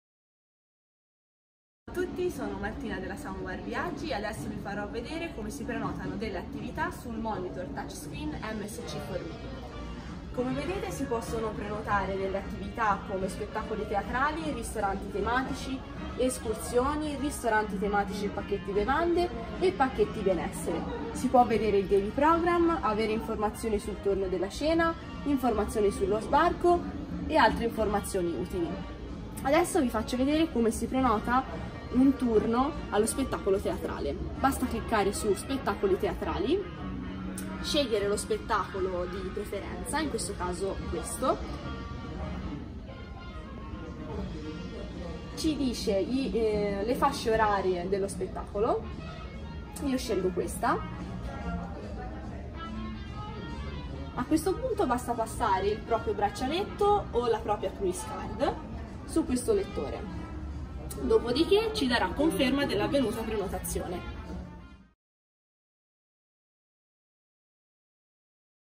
Come prenotare un posto al teatro dal monitor Msc For Me-Msc Crociere
In questo video vi mostreremo come si prenota un posto al teatro tramite il monitor touch screen Msc For Me a bordo di Msc Grandiosa